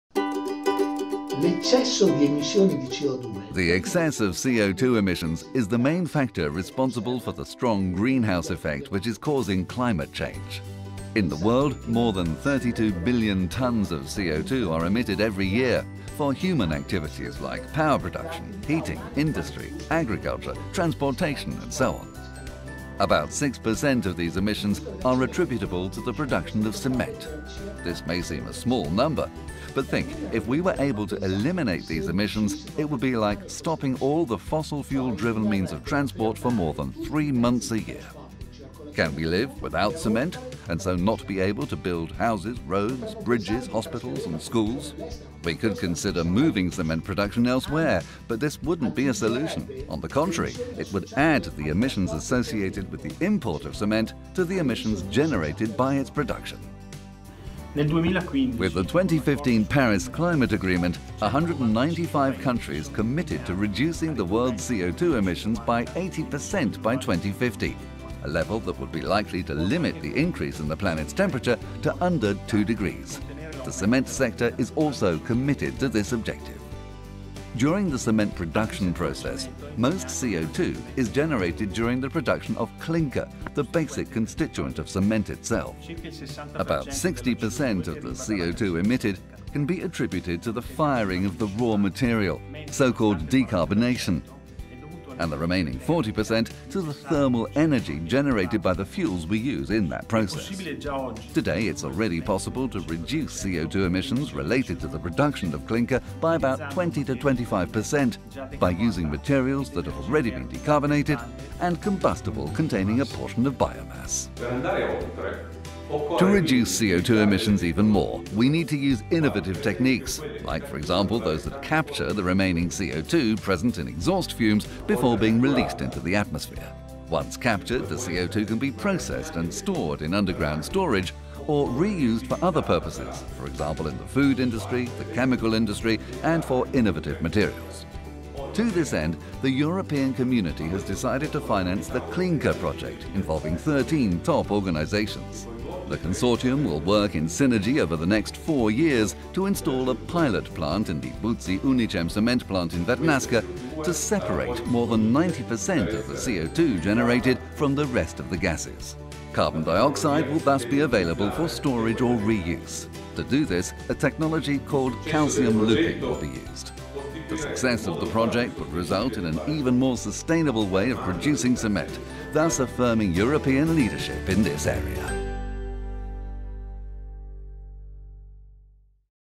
CLEANKER, CLEAN clinKER production by calcium looping process
The cement industry can play a key role in the reduction of CO2 emissions. CO2 generation in the cement production is mainly related to the calcination of limestone (CaCO3 dissociated to CaO and CO2), the main raw material used in the production process. Around 60% of CO2 emissions come from this process reaction.
In addition, fossil fuels combustion emissions are relevant as well; also, on top of direct CO2 emissions, the generation of electric power required by the process (e.g. grinding) is responsible for indirect CO2 emissions.
There are currently no feasible methods to produce clinker, and thus cement, without releasing CO2 from CaCO3, and, given the lifetime of a cement plant (30-50 years), the technologies to be developed will have to be retrofits.
In addition to oxyfuel combustion and post-combustion solvent-based capture technologies, which so far attracted most of the research efforts , Calcium Looping is recognized as another very promising emerging technology for CO2 capture.
The ultimate objective of CLEANKER is demonstrating the applicability of the Calcium Looping (CaL) to the cement production process, with the usage of entrained flow reactors, in order to produce low-CO2 clinker. A complete demonstration system will be installed by the 1300kton/year cement plant, operated by Buzzi Unicem, in Vernasca (Piacenza, Italy).
The project is led by LEAP and has received funding from the European Union's Horizon 2020 research and innovation programme under grant agreement No 764816.